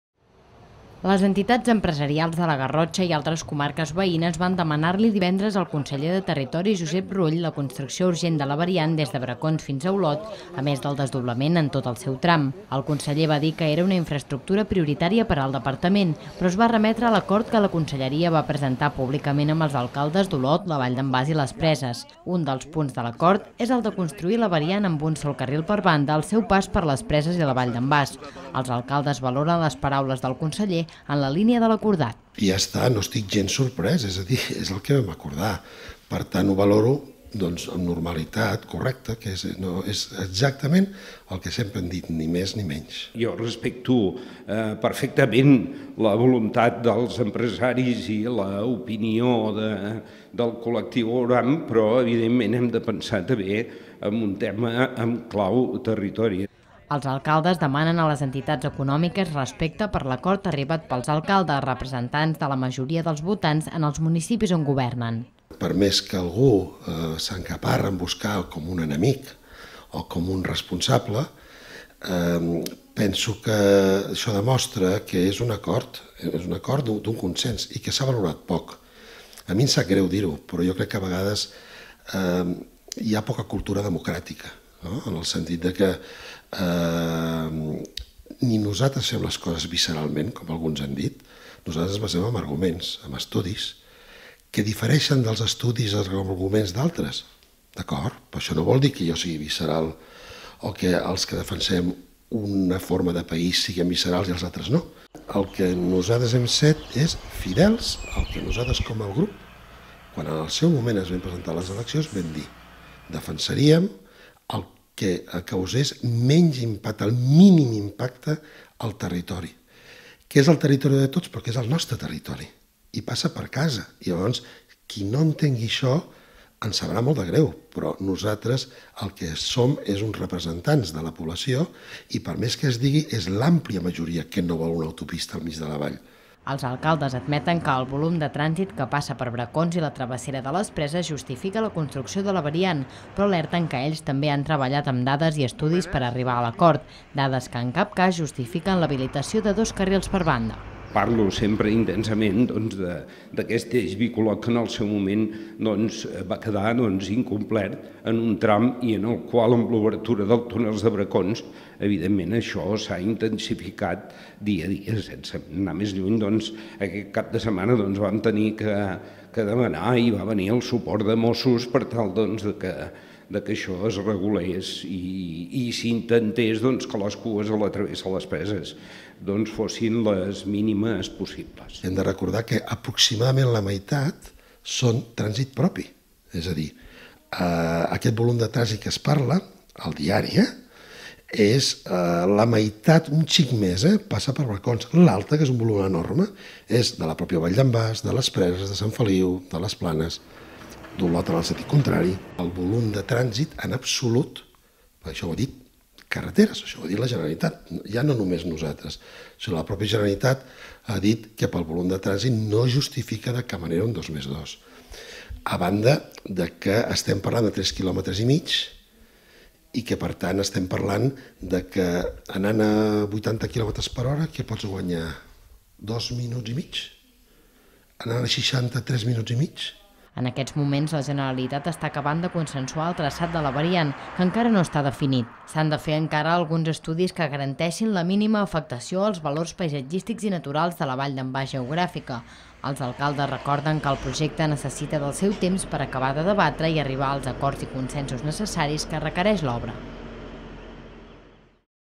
Els alcaldes demanen a les entitats econòmiques respecte per l’acord arribat sobre la variant
Els alcaldes de la Vall d’en Bas i les Preses valoren sense sorpresa la resposta del conseller de Territori a les entitats empresarials, quan es va remetre a l’acord establert amb els Ajuntaments. Els alcaldes demanen respecte per l’acord pres a quatre bandes entre els ajuntaments i la Generalitat per construir una variant el més respectuosa amb els valors paisatgístics i mediambientals de la Vall d’en Bas.